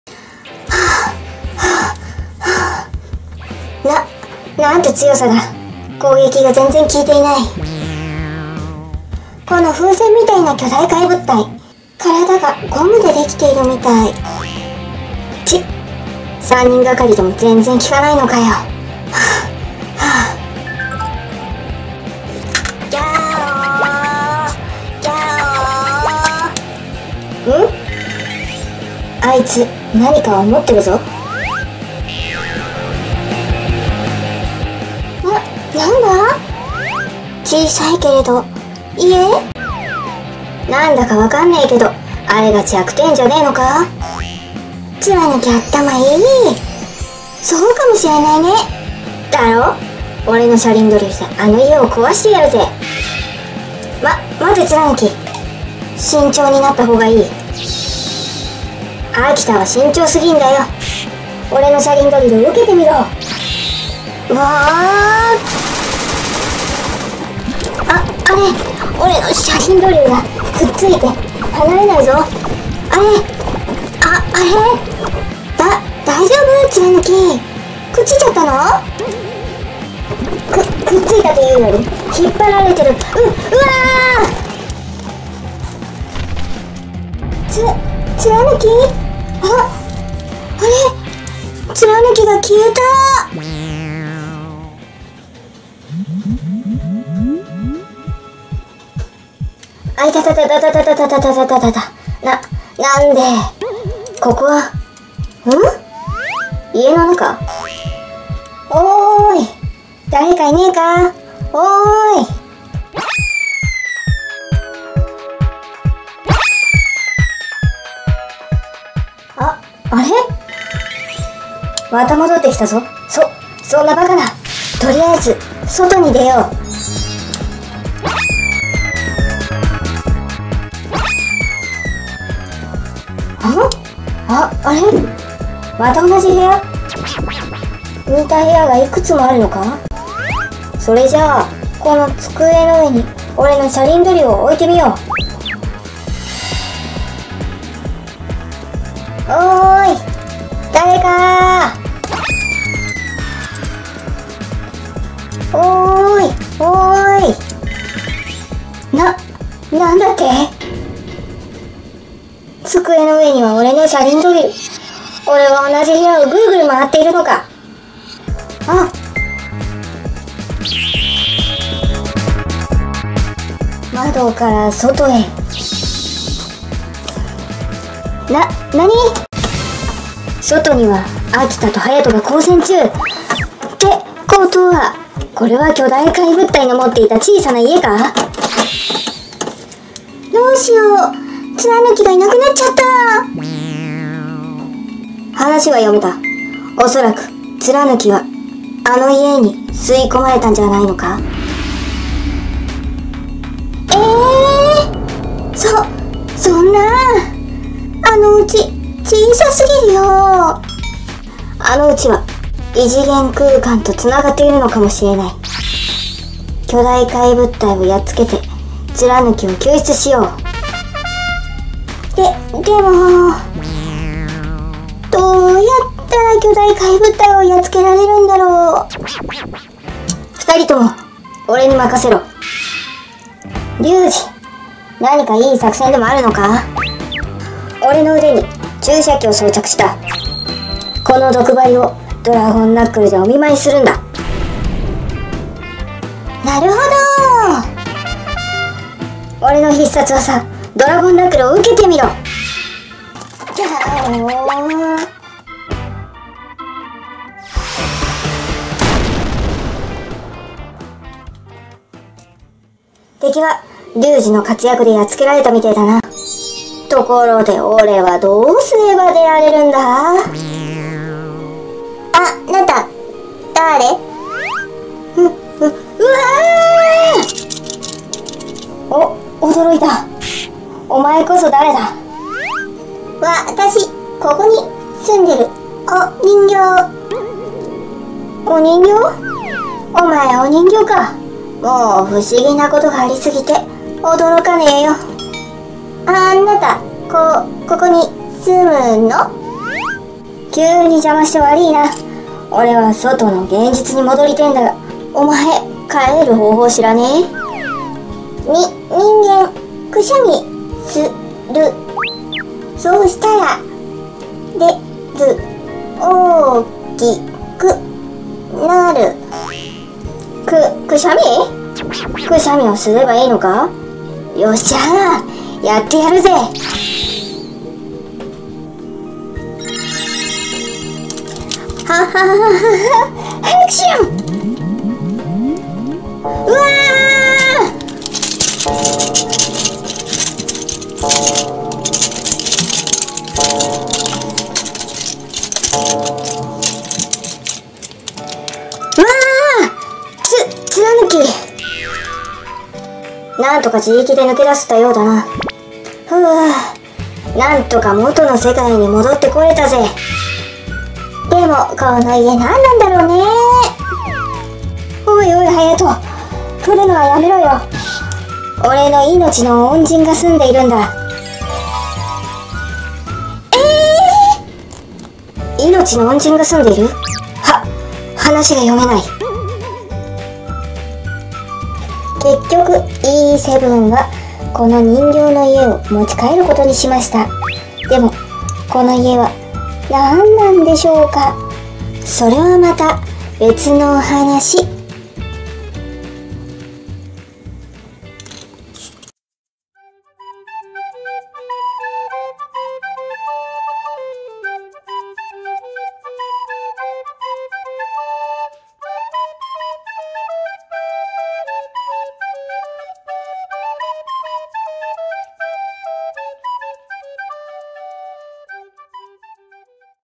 不思議な家に吸い込まれた!!シンカリオンＥ７かがやき に襲い掛かる不思議な出来事！閉じ込められたツラヌキは脱出することができるのか？プラレール ピンキーパンキー[PINKYPUNKY]
【新幹線変形ロボ シンカリオン】シンカリオンたちが巨大怪物体を攻撃しますが、敵を倒すことが出来ません。風船のような巨大怪物体をよく見ると、なぜか体に小さな家がくっついているではありませんか。ツラヌキがその家を攻撃しようと言い出しました。
シャリンドリルで一突きしようと試みますがまったくきかず、それどころか家にグイグイ引きずり込まれていきます。
気が付けばツラヌキはその不思議な小さな家に吸い込まれていました。
小さな家の窓から外を眺めるとハヤトたちが戦っているのが見えました。
いったいどうやったらこの家から抜け出すことができるのでしょうか？

ピンキーパンキー[PINKYPUNKY]です。

ピンキーパンキー[PINKYPUNKY]

毎日更新！再生リスト